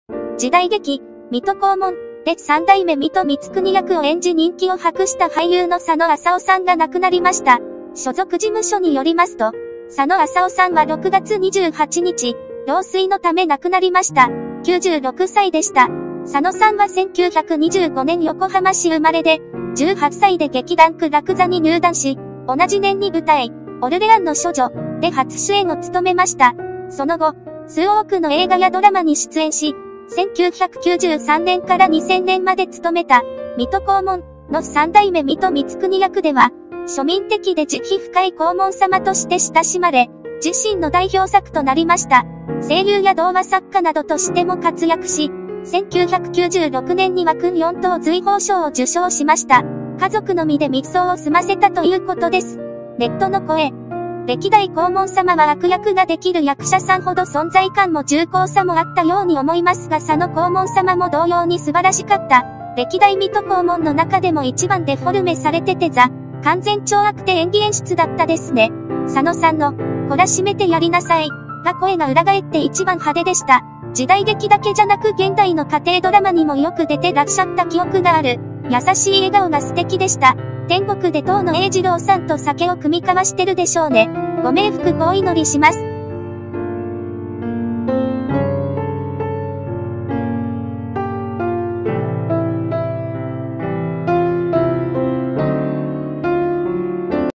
【訃報】俳優 佐野浅夫さん（96）老衰で死去 水戸黄門で3代目水戸光圀役
時代劇「水戸黄門」で3代目水戸光圀役を演じ人気を博した俳優の佐野浅夫さんが亡くなりました。

　所属事務所によりますと、佐野浅夫さんは6月28日、老衰のため亡くなりました。96歳でした。

　佐野さんは1925年横浜市生まれで、18歳で劇団苦楽座に入団し、同じ年に舞台「オルレアンの処女」で初主演を務めました。

　その後、数多くの映画やドラマに出演し、1993年から2000年まで務めた「水戸黄門」の3代目水戸光圀役では、庶民的で慈悲深い黄門様として親しまれ、自身の代表作となりました。

　声優や童話作家などとしても活躍し、1996年には勲四等瑞宝章を受賞しました。

家族のみで密葬を済ませたということです。。